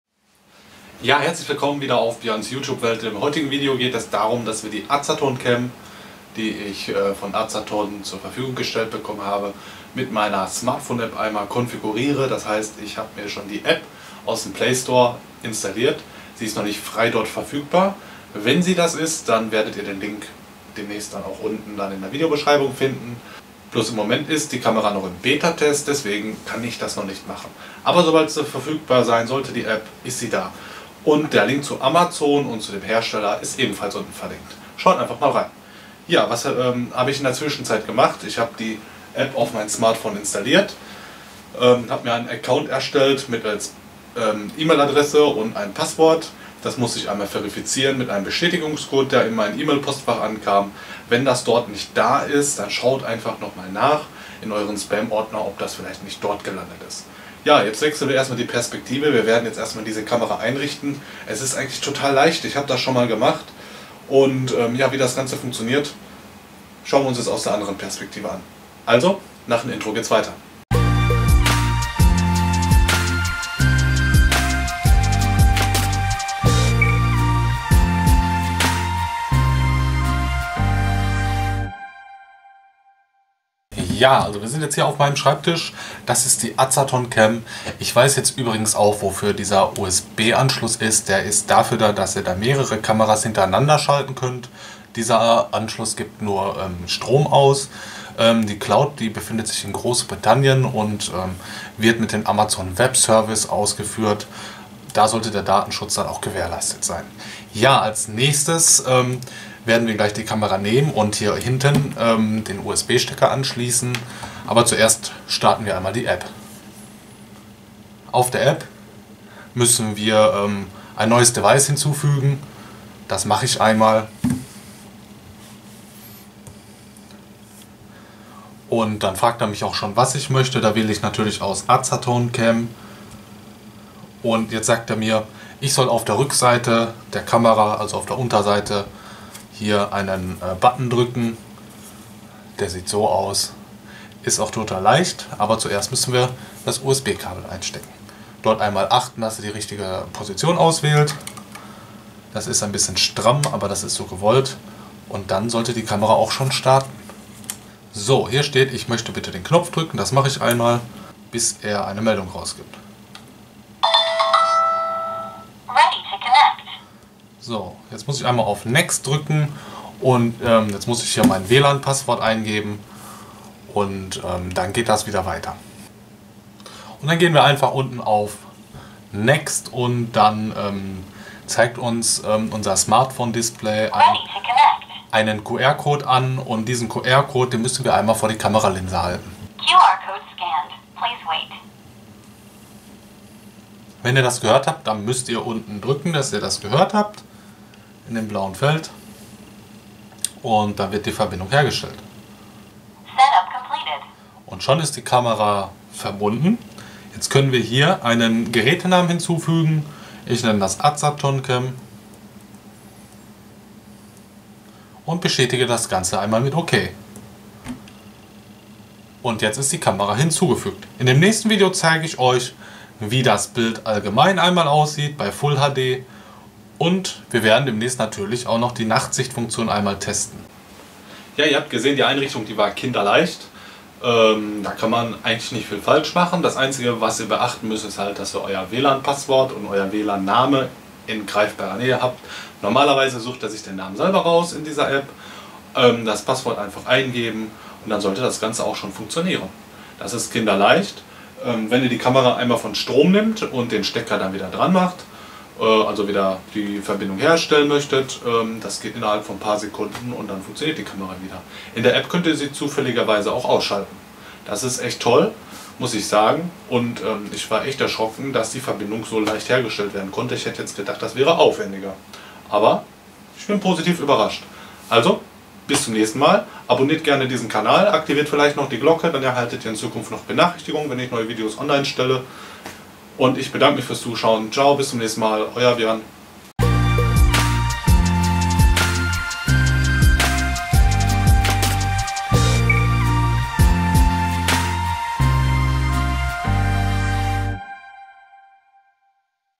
Smart Home Kamera von Azarton einrichten Deutsch Alexa und Google Home Erweiterungsgerät 2 Cloud AWS
In diesen Video zum Thema Smart Home zeige ich euch die Einrichtung der App für die Azarton Cam. Diese Smart Home Kamera wird über die AWS Cloud in der EU betrieben. Die Einrichtung der App für die Smart Home Kamera ist kinderleicht. Die Full HD Bildqualität ist echt super, und das Ergebnis der Azarton Cam kann sich sehen lassen. Da ich schon weitere Smart Home Geräte gezeigt habe ist diese Kamera eine super Ergänzung.

Hier meine wichtigsten Links:
Vielen Dank fürs Zuschauen und wir hoffen, dass Ihnen das Video gefällt!